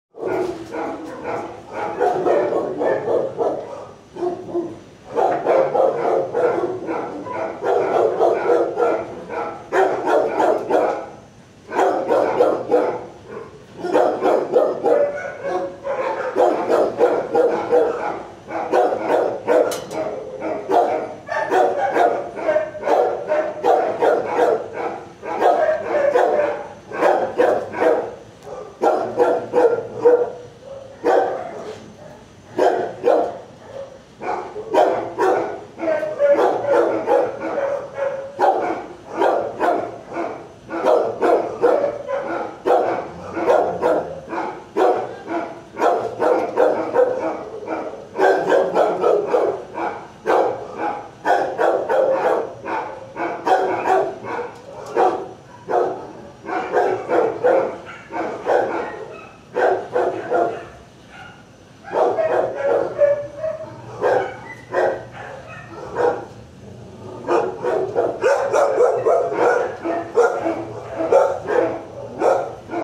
Honey 2y A839774 Buddy 6y A695079 kennel 175 PACC shelter Honey unnerved by my back kennel intro 660
Buddy seemed okay with my presentation but that was a little too peculiar for Honey. Live and learn.